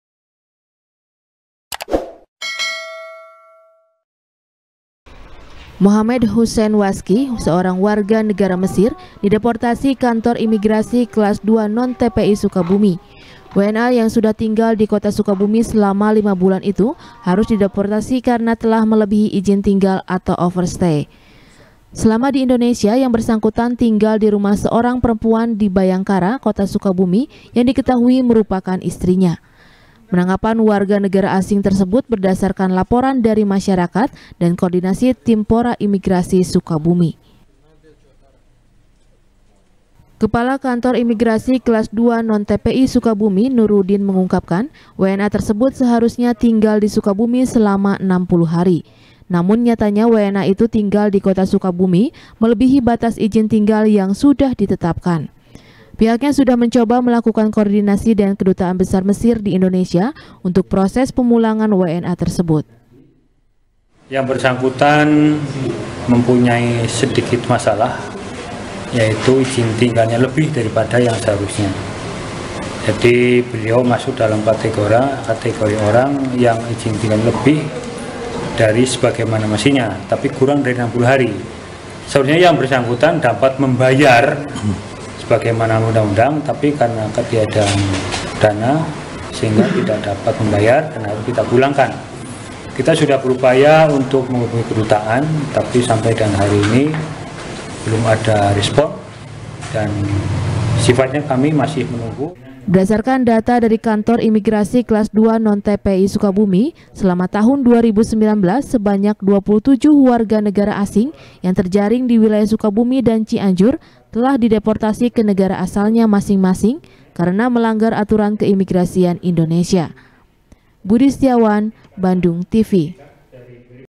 Melebihi Ijin Tinggal, Warga Negara Mesir Di Deportasi Imigrasi Sukabumi | SBR | BANDUNGTV
SBR | BANDUNGTV | Kantor Imigrasi Kelas II Non TPI Sukabumi Menangkap Seorang Warga Negara Asing Asal Mesir  Karena Diduga Melanggar Aturan Keimigrasian Yakni Melebihi Ijin Tinggal. WNA Tersebut Kini Ditahan Di Ruang Detensi Imigrasi Dan Dalam Proses Pemulangan Atau Deportasi Ke Negara Asalnya.,